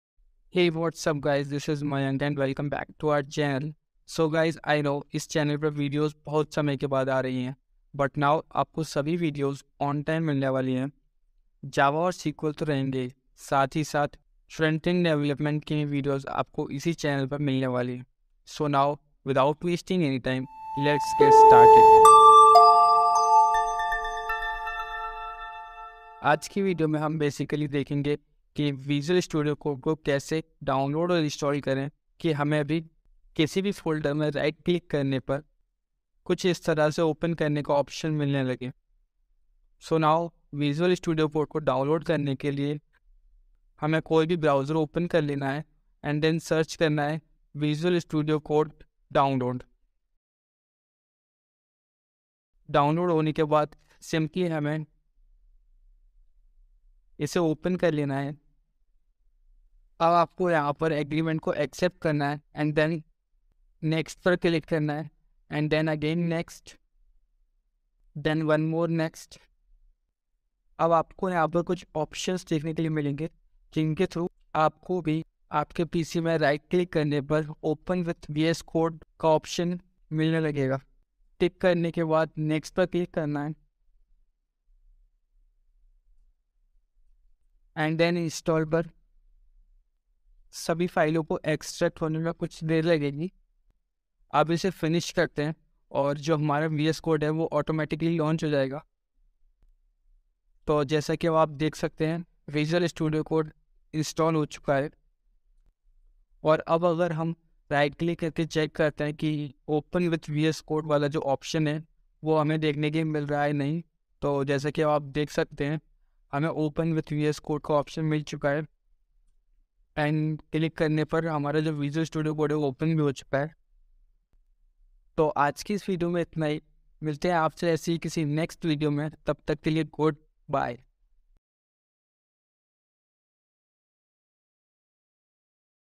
Install VS Code In 2 Minutes | How to Install and Set up Visual Studio Code for Programming
In this video, we will walk you through the process of installing Visual Studio Code (VS Code) on your computer. VS Code is a popular, free, and open-source code editor developed by Microsoft. It is widely used by developers for writing and editing code in various programming languages. We will cover the installation process for Windows operating systems. The video will also show you how to verify that VS Code has been installed correctly and how to launch the application. By the end of the video, you will have a working installation of VS Code on your computer, ready for use.